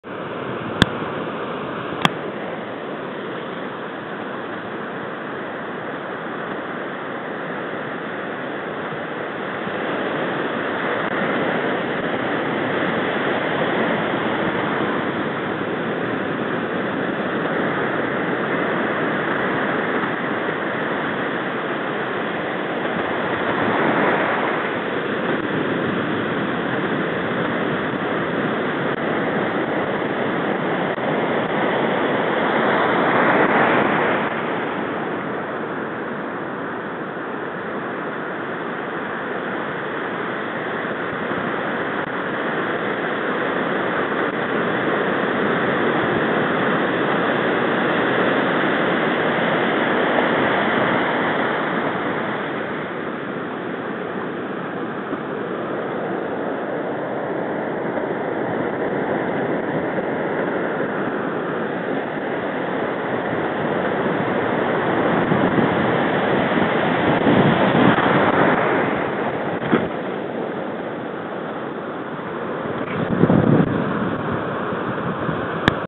Surf fly fishing
A 12 inch surf perch on the red checkerboard fly